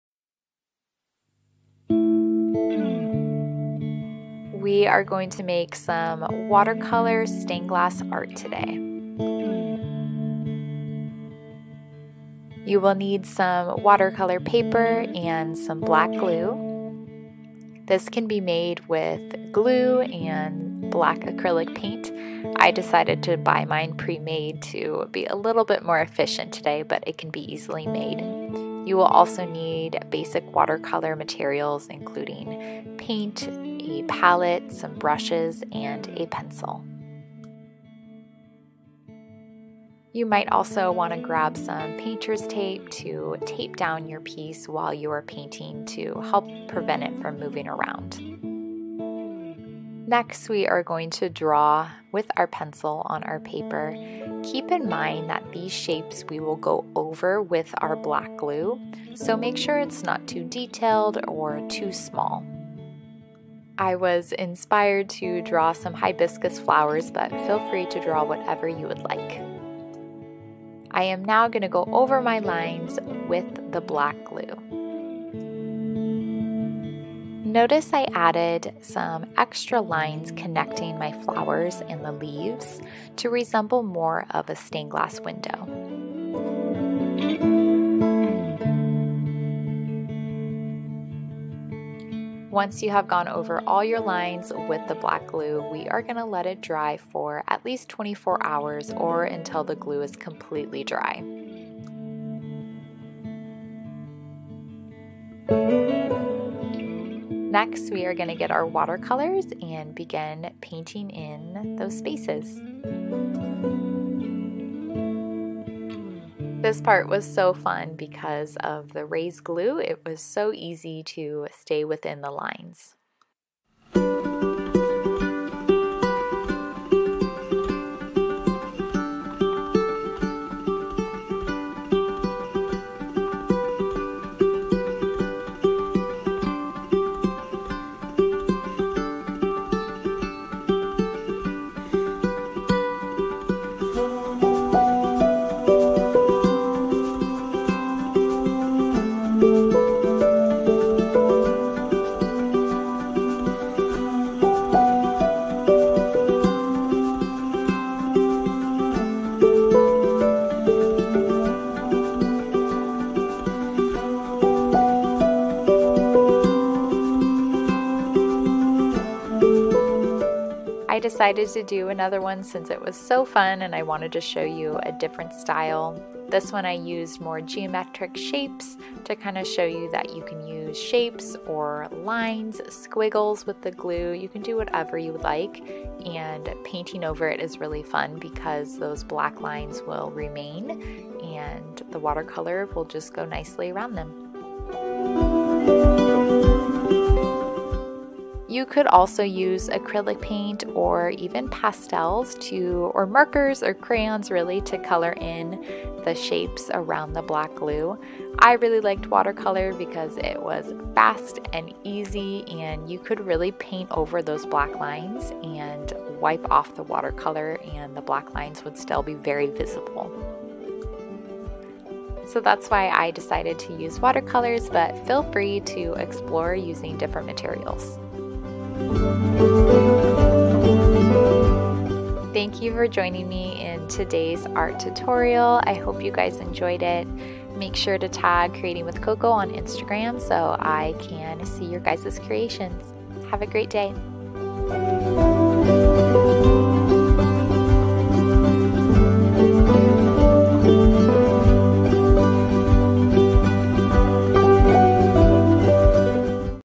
Watercolor Stained Glass Art
Learn how to create stained glass inspired art with black glue and watercolor! This project may take up to 3 days to make. Please factor in the time it will take for your glue to dry before adding in watercolor.

Materials needed for this project:
Black glue

Make sure to follow along @creatingwithcoco on instagram to view my other painting projects and tag me so I can see your creations!